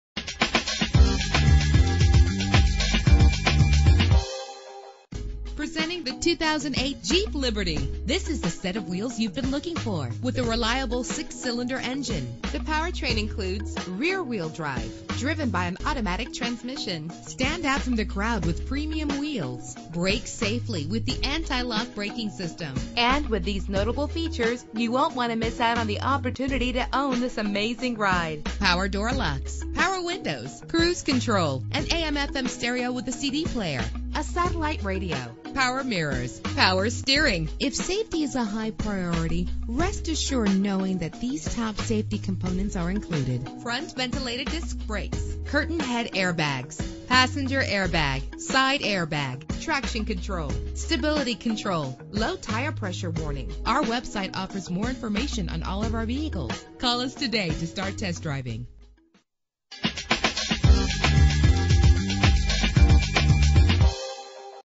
2008 Jeep Liberty - Carlsbad CA
Year: 2008
Make: Jeep
Model: Liberty
Trim: Sport
Engine: 3.7 liter 6 cylinder 12 valve
Transmission: 4-Speed Automatic
Color: Light Gray
Mileage: 46692
Address:
5555 Car Country Dr